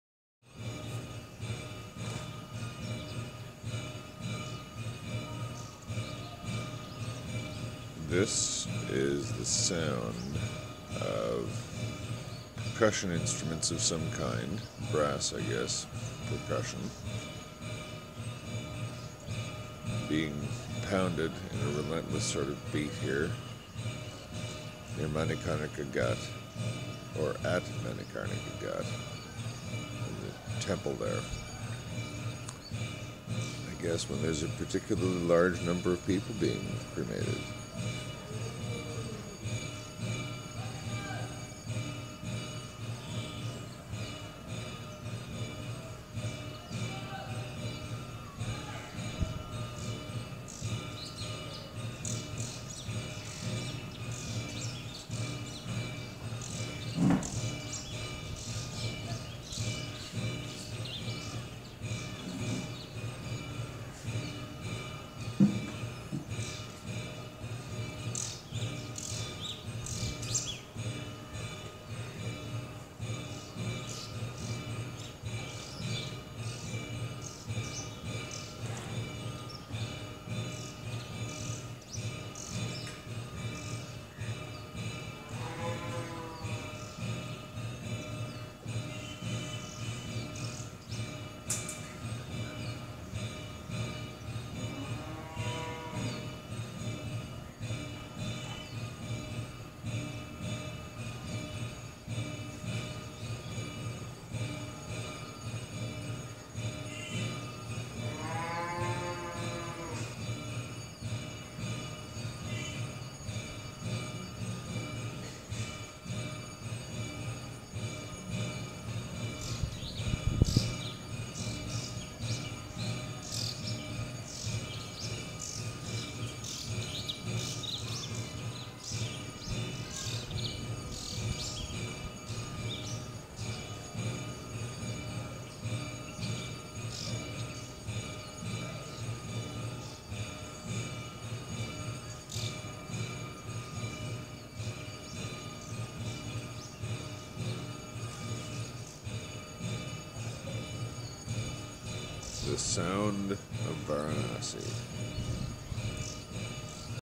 Death As Triumph.
Albeit a nervous sort of triumphalism. The bells - if, indeed, they are bells - seem joyous, but acutely aware of their own profundity.

This is death. About as profound as it gets. Dawn at Manikarnika Ghat, Varanasi, India.

The European-looking spire at lower centre is where the cremations take place.